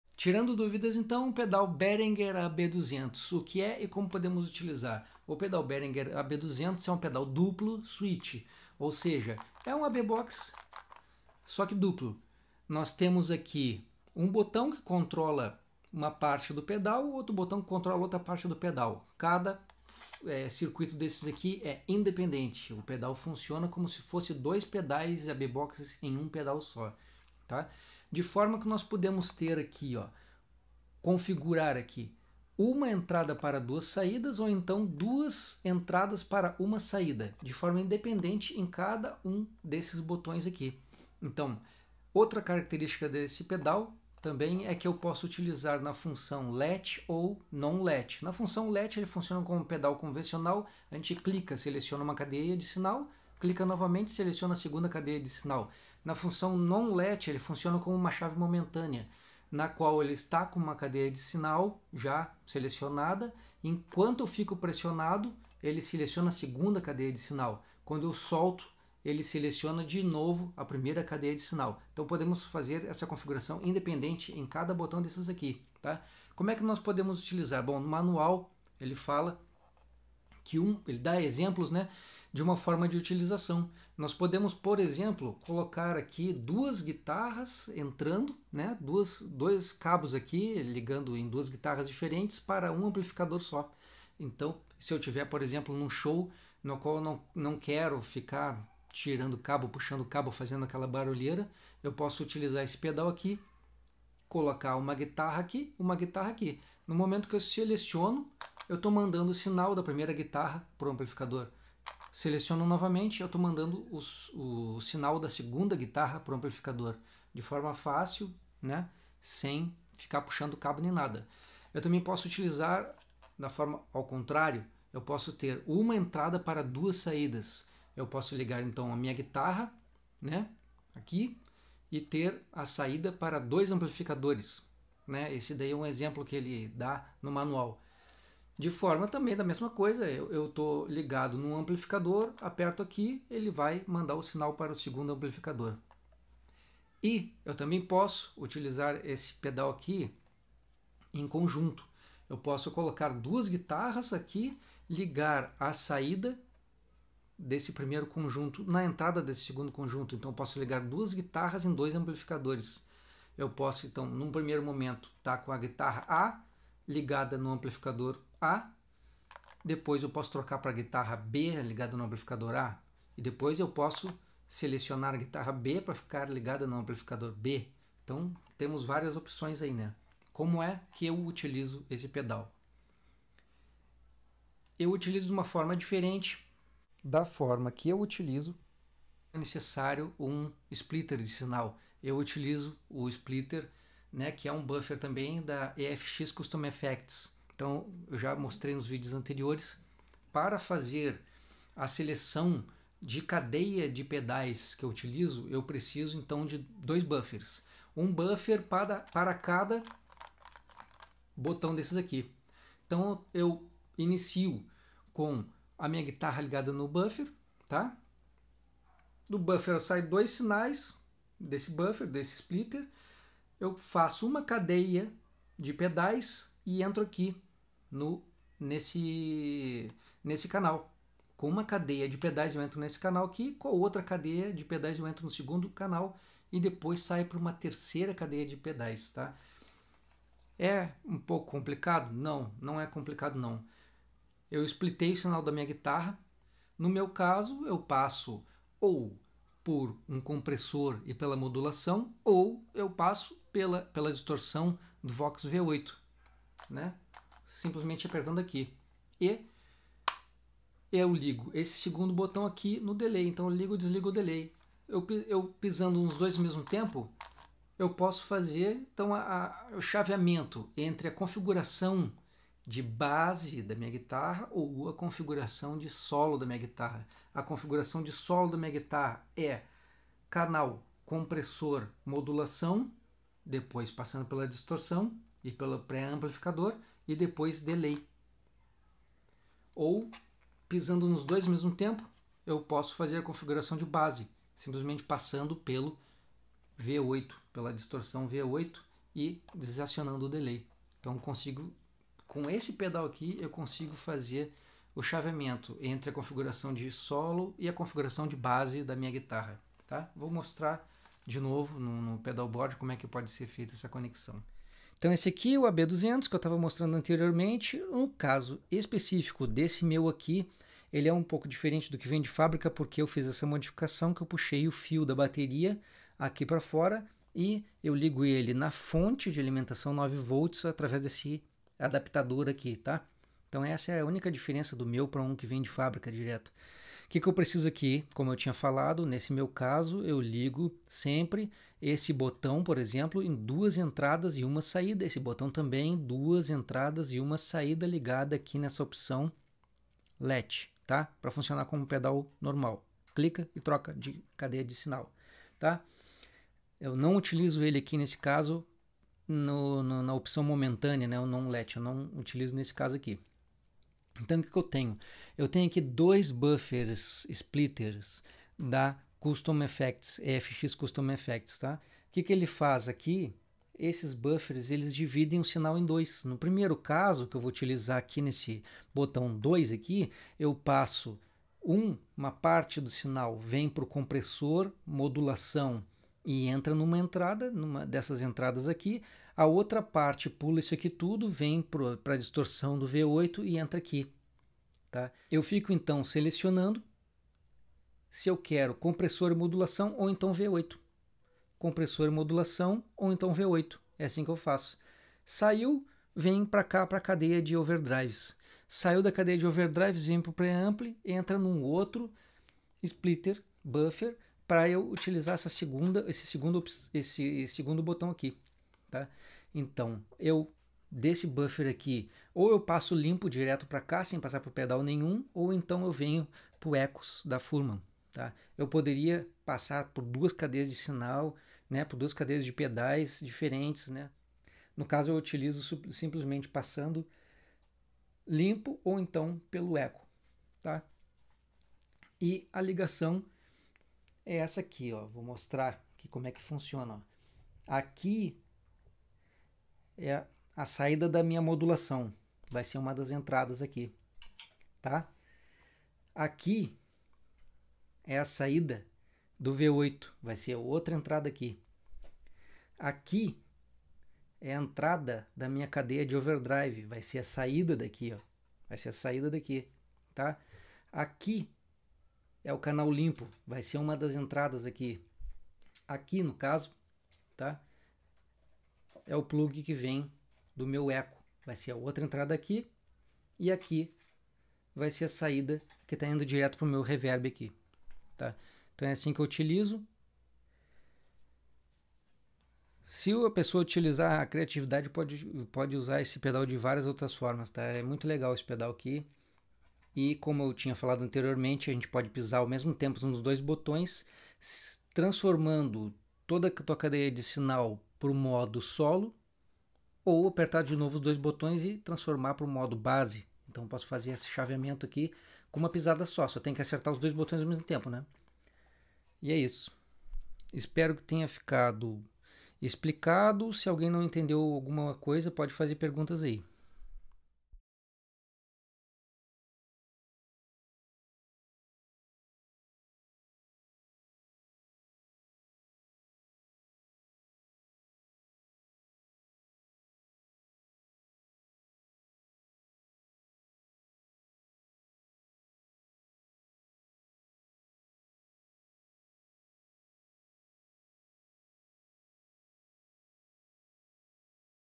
Pedal Behringer AB200
Respondendo a comentários do canal: como usar o Behringer AB200